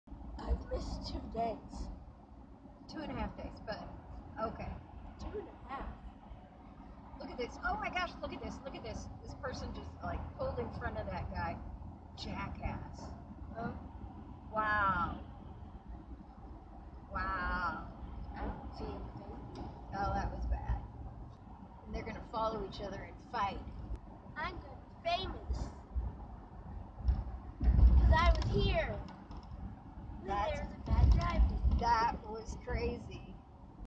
Dash Cam Bad Drivers - 1 car cuts off another
Overland Park Kansas - My wife and daughter are driving home when they witnessed a near miss as one car pulls in front of another.  The event doesn't appear that bad on video but I do love their reaction and conversation about it.